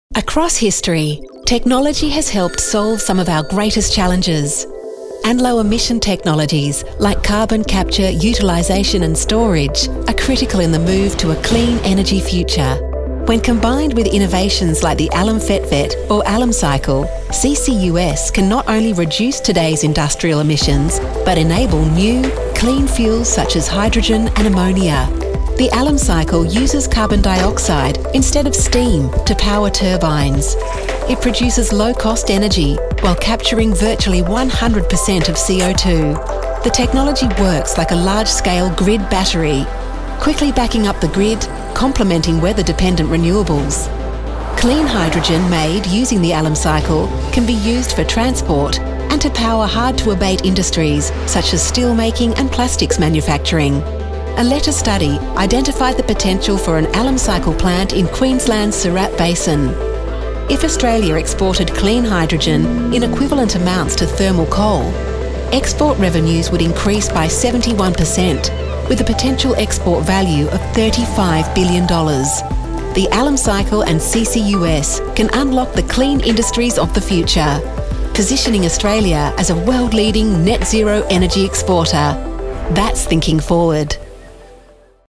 A Clean Energy Future with the Allam Cycle and CCUS
The Allam Cycle and carbon capture utilisation and storage can work together to unlock the clean energy and industries of the future. Learn how.